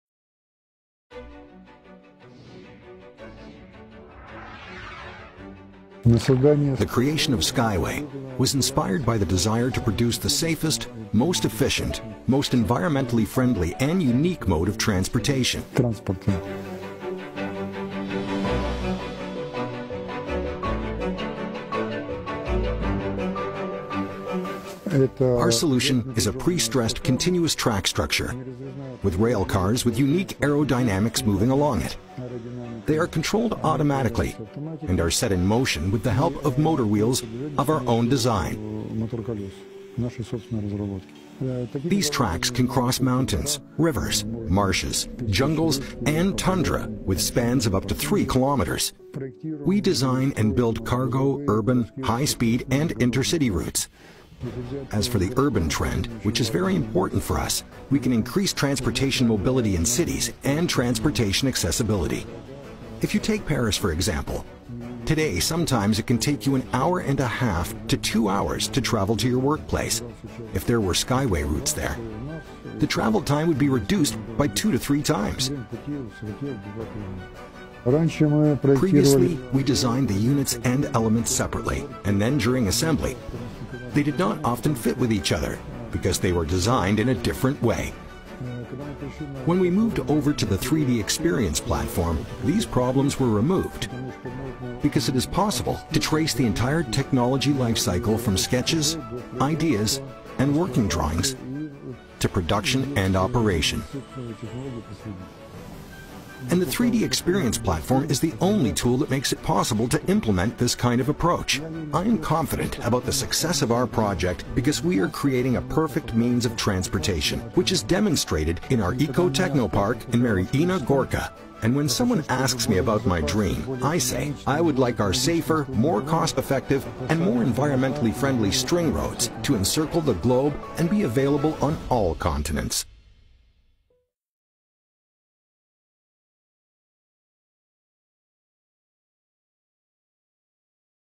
𝗟𝗲 𝗽𝗮𝗿𝘁𝗲𝗻𝗮𝗶𝗿𝗲 𝗱𝗲 𝗨𝗡𝗜𝗧𝗦𝗞𝗬 𝗦𝗧𝗥𝗜𝗡𝗚 𝗧𝗘𝗖𝗛𝗡𝗢𝗟𝗢𝗚𝗜𝗘𝗦
« Nous créons un nouveau système de  transport majeur  (Skyway)
respectueux de l'environnement, sûr, confortable et louable à la fois dans la production et l'exploitation par rapport aux autres solutions existantes à traver SWC platform